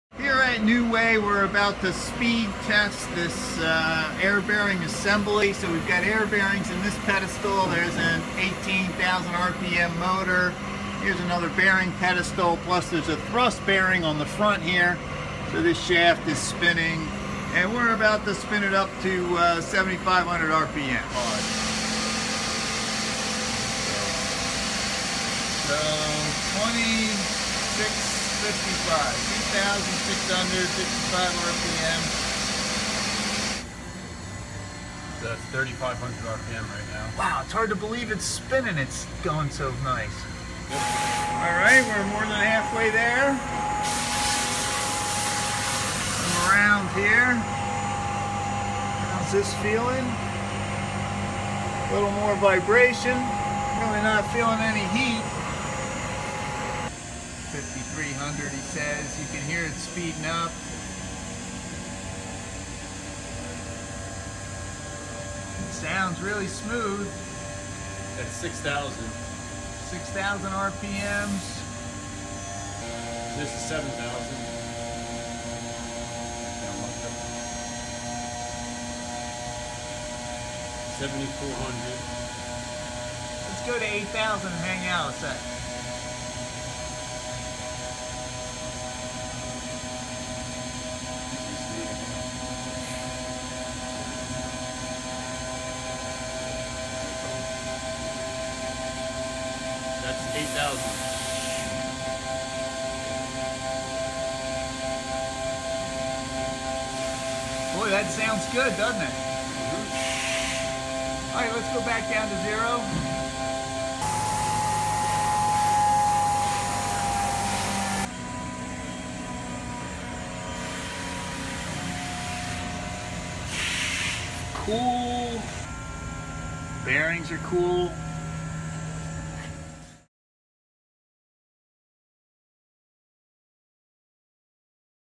Thrust Bearing As Seal 8000 RPM
In this video, Drew Devitt, CTO at New Way Air Bearings shows how a thrust bearing can be used in a sealing application in Turbomachinery.